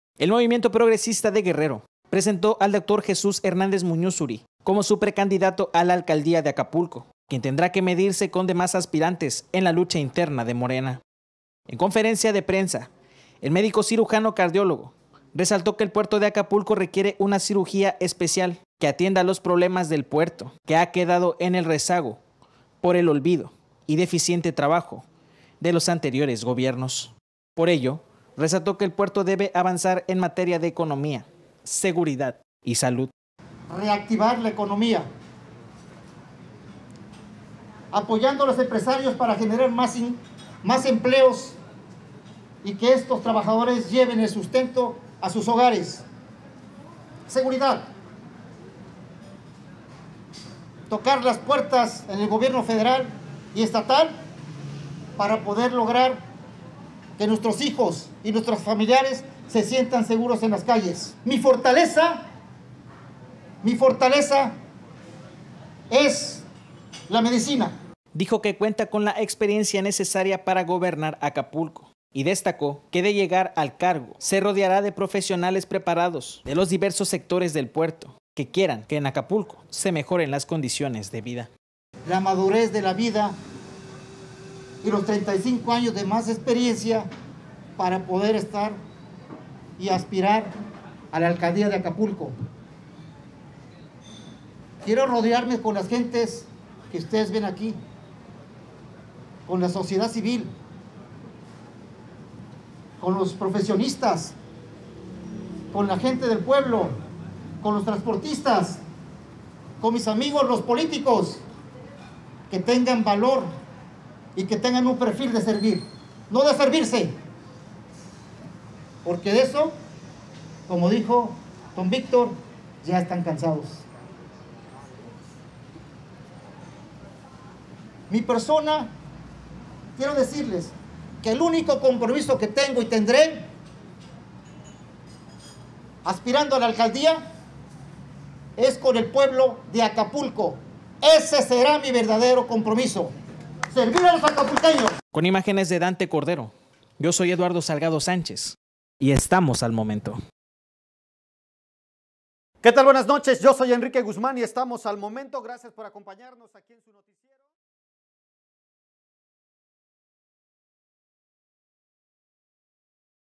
Presentan al Dr Muñuzuri como Precandidato a la Alcaldía de Acapulco
Fue presentado de manera ex terna el médico cirujano cardiólogo, quien se ha desempeñado en el campo de la medicina en el estado de Guerrero.